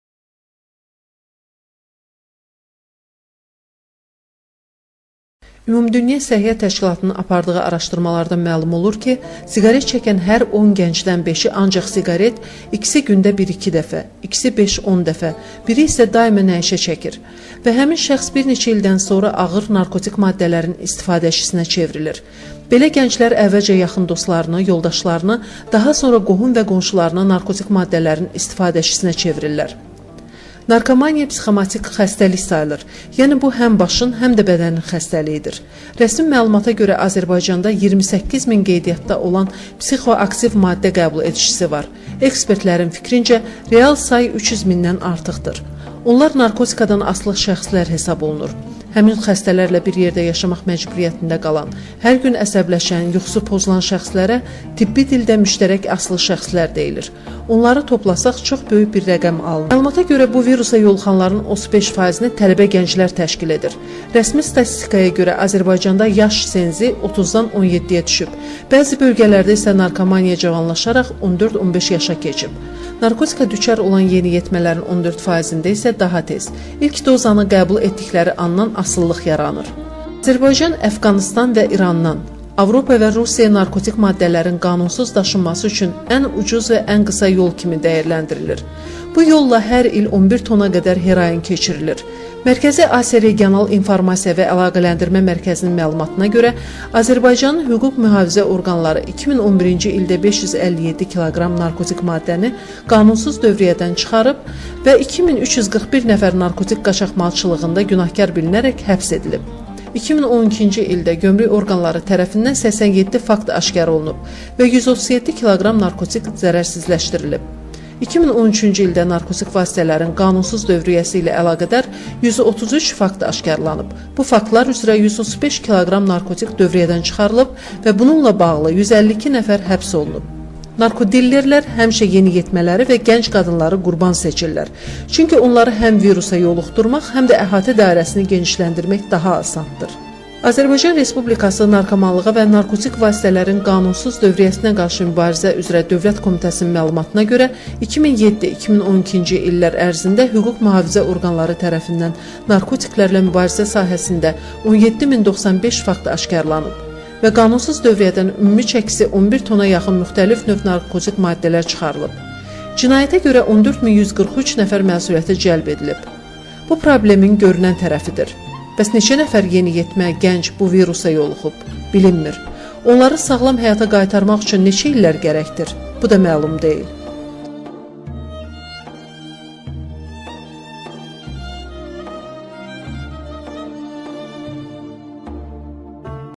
Tərtər rayon Dəmirçilər kənd Sənətkarlıq evi Narkomaniya ağ ölümdür adlı videoçarx hazırlayıb
Tərtər rayon Mədəniyyət Mərkəzinin tabeliyində olan  Dəmirçilər kənd Sənətkarlıq evi Narkomaniya ağ ölümdür adlı videoçarx hazırlayıb.